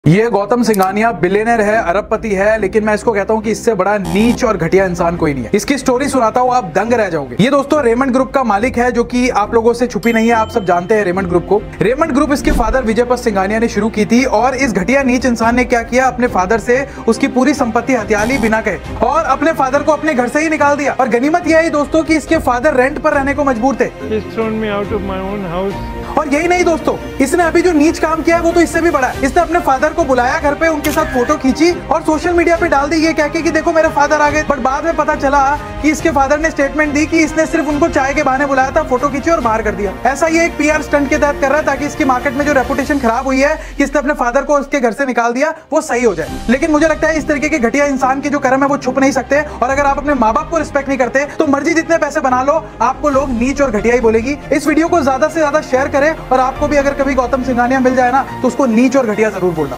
Exposing Billionaire | Raymond Gautam Singhania
In this video we will exposing Gautam Singhania, who is a billionaire from Raymond group.